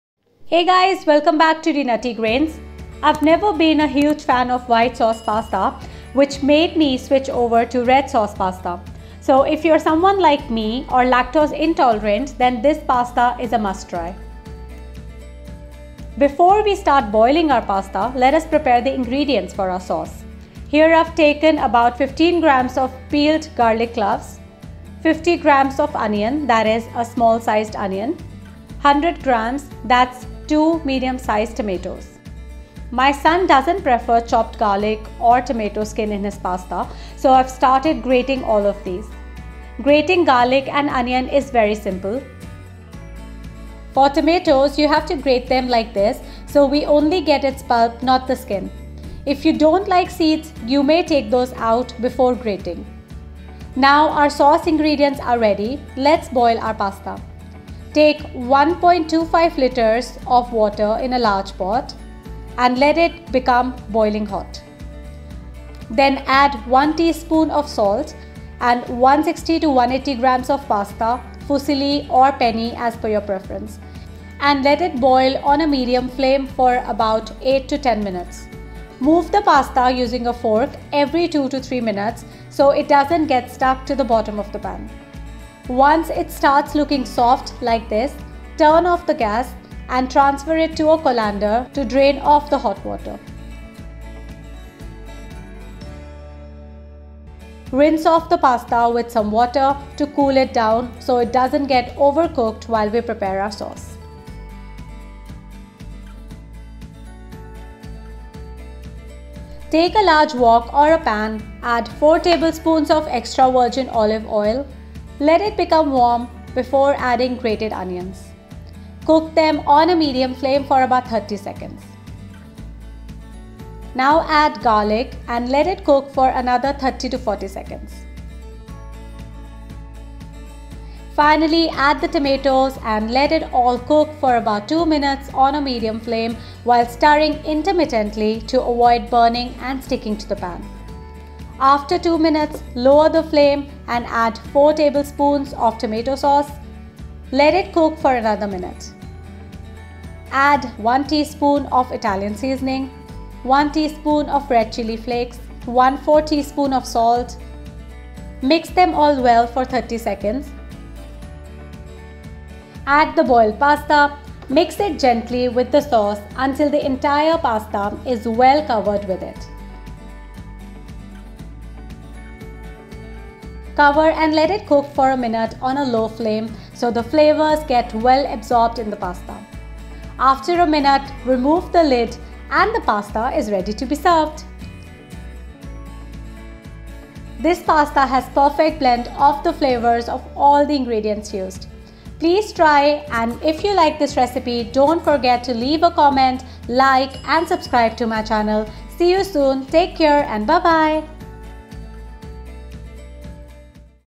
Red Sauce Pasta | Gluten Free | Lactose Free | Great Breakfast Option
Kitchen-aid hand whisk
Kitchen-aid measuring cups
Mixing bowls

Ingredients:

Fusilli or Penne pasta: 160-180gms
Water: 1.25 ltrs

Garlic  : 15gms
Onion   : 50gms
Tomatoes: 100gms

Extra Virgin Olive Oil : 4 tbsps
Ketchup : 4 tbps

Italian Seasoning: 1 tsp
Red Chilli flakes: 1 tsp
Salt : 1/4 tsp

Hey guys, welcome back to D'Nutty Grains...I have never been a huge fan of white sauce pasta, which made me switch over to Red Sauce Pasta..So, if you are someone like me, or lactose intolerant, then this pasta is a must try...

Before we start boiling our pasta, let us prepare the ingredients for our sauce...Here, i have taken about 15gms of peeled garlic cloves, 50 gms of onion, i.e., a small size onion; 100gms, that's 2 medium sized tomatoes... My son doesnt prefer chopped garlic of tomato skin in his pasta, so i have started grating all of these...Grating garlic and onion is very simple. For tomatoes, you have to grate them like this, so we only get its pulp, not the skin...If you don't like seeds, you may take those out, before grating...Now, our sauce ingredients are ready, let's boil our pasta...

Take 1.25 ltrs of water in a large pot, and let it become boiling hot. Then add 1 tsp of salt and 160-180 gms of pasta: fusilli or penne, as per your preference, and let it boil on a medium high flame for about 8-10mins. Move the pasta using a fork, every 2-3mins, so it doesnt get stuck to the bottom of the pan... Once it starts looking soft, like this, turn of the gas, and transfer it to a colander, draining off the hot water...Rinse off the pasta, with some water, to cool it down, so it doesnt get overcooked while we prepare our sauce..

Take a large wok or a pan, add 4 tbsps of extra virgin olive oil...let it become warm, before adding grated onions...cook them on a medium flame for about 30secs...then add garlic, let it cook for another 30-40secs...Finally add the tomatoes, and let it all cook for about 2mins on the medium flame, while stirring intermittenly, to avoid burning and sticking to the pan...After 2mins, lower the flame and add 4 tbsps of tomato sauce, let it cook for another min. Now add 1/4 tsp of salt, 1 tsp of italian seasoning, 1 tsp of red chilli flakes...Mix them all well for 30 secs...then add pasta...mix it gently with the sauce until the entire pasta is well covered with it...Cover and let it cook for a minute on a low flame, so the flavours get well absorbed in the pasta...After 1 minute, remove the lid, and the pasta is ready to be served.